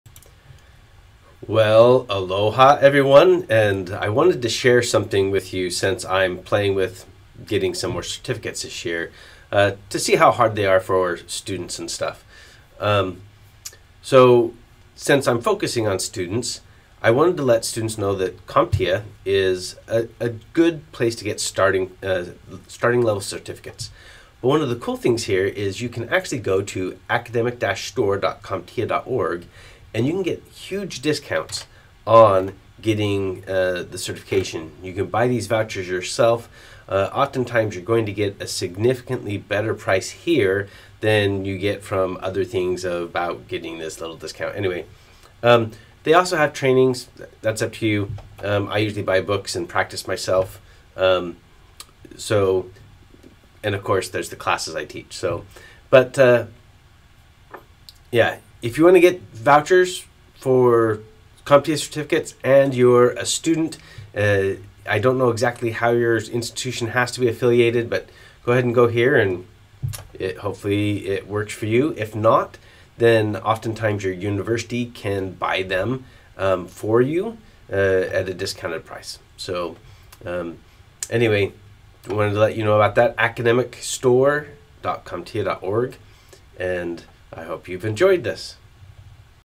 Students purchase discounted certification vouchers from the Academic CompTIA Store
Students can buy certification vouchers for CompTIA certifications at a steep discount If you are looking to get started with some certifications, this is a good place to start.